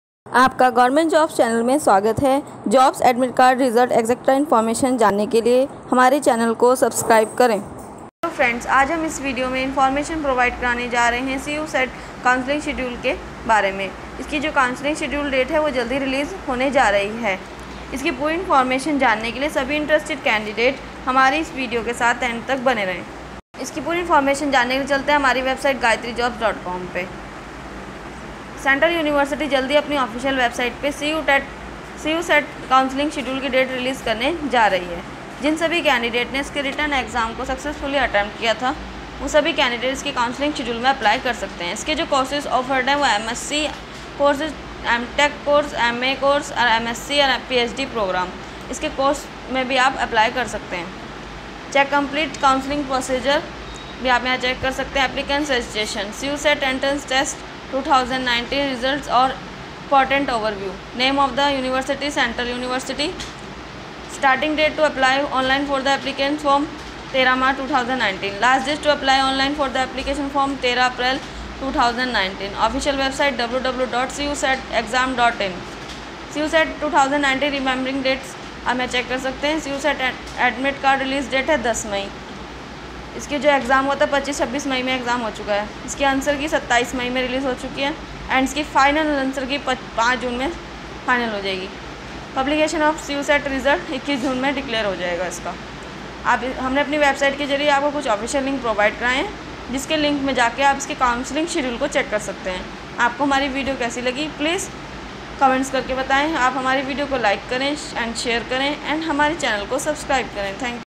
CUCET Counselling 2019 Schedule Dates for Admission in PG PHD Courses
Central Universities will soon release the CUCET Counselling 2019 for Integrated M.Sc (5 Yrs) Courses, M.Tech Courses, M.A Courses, M.Sc Courses, Ph.D. Courses. Check & Download CUCET Seat Allotment Result 2019 @